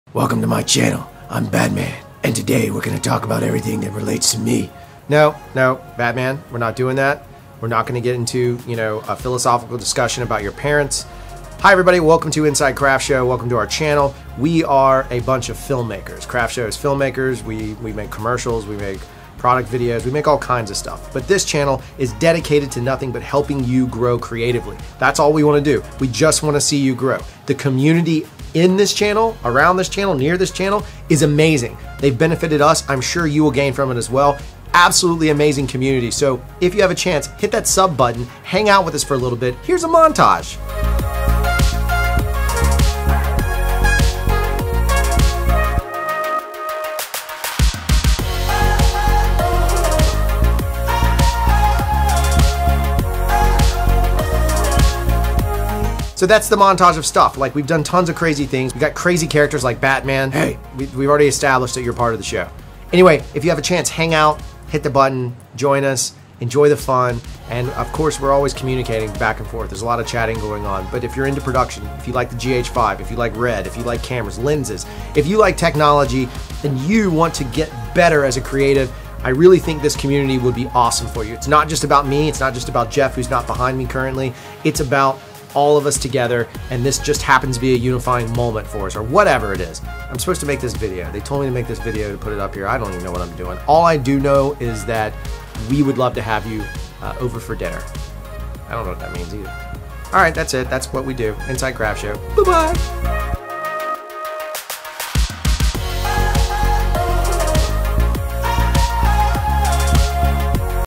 Get Inside CRFTSHO - boost creativity, learn tricks and tips, be in awe of Jeff's beard
We are CRFTSHO...but this is our channel. We are filmmakers, creative professionals, and fans on inspiration. Our goal with this channel is to offer creative insight from both our community and our experiences. If you're into cameras, production, and being creative we'd love to have you hang out.